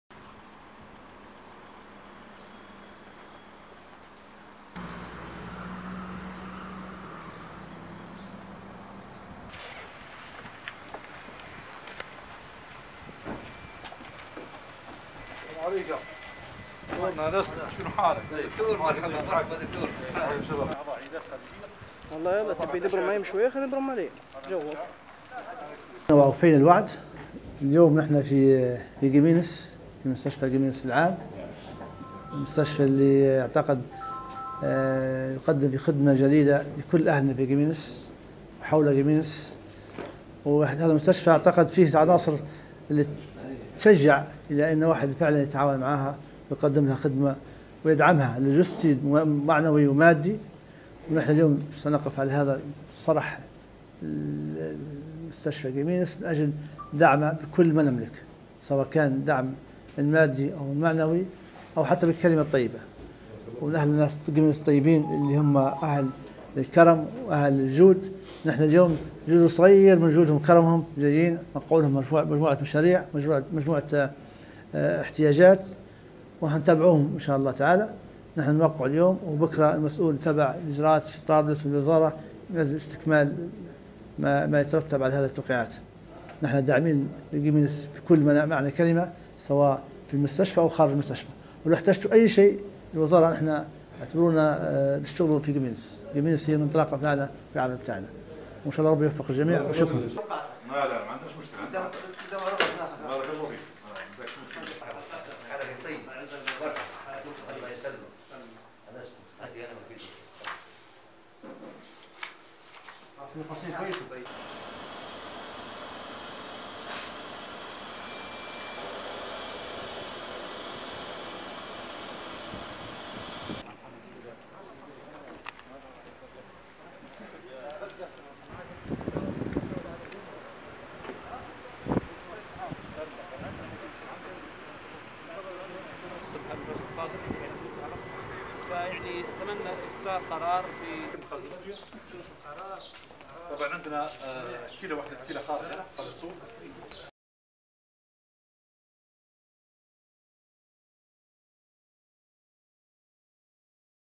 زيارة وزير الصحة إلى مستشفى قمينس العام والوحدات الصحية التابعة له LCNA
وكالة الغيمة الليبية للأخبار - بنغازي.
قام وزير الصحة بحكومة الوحدة الوطنية "علي الزناتي" بزيارة تفقدية لمستشفى قمينس العام والوحدات الصحية التابعة له، حيث شملت الزيارة جميع الأقسام بالمستشفى والاطلاع على جاهزية المستشفى لقرب افتتاح العيادات الخارجية بعد أن تم إكمال مرحلة الصيانة وذلك بعد عيد الفطر المبارك حسب توجيهات الوزير.